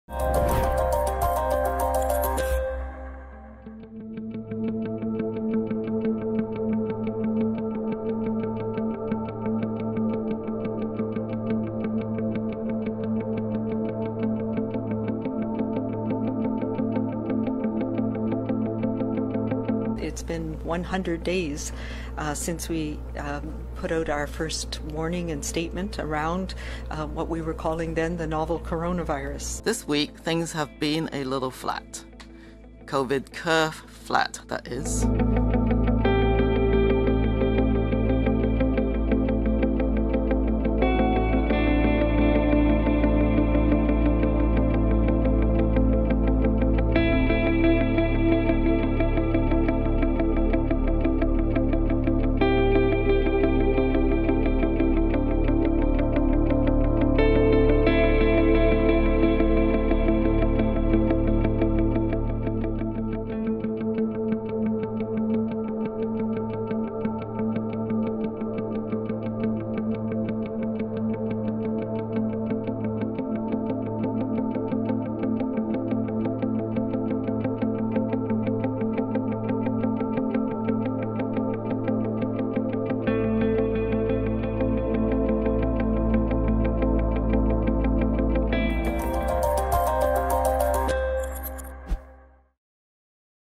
Here's What's Making News April 30, 2020
From the ongoing search for Canadian military personnel aboard a crashed helicopter, to B.C. marking 100 days since its first warning about COVID-19, here are some of the stories at citynews1130.com and live on-air at player.news1130.com